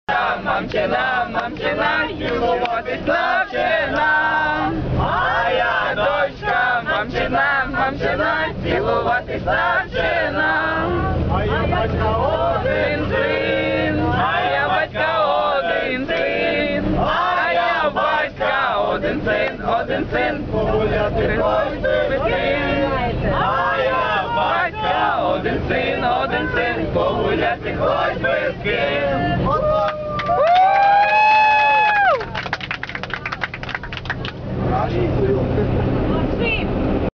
Как нужно ездить в автобусах)
Funny crazy trip from Hungary back to Ukraine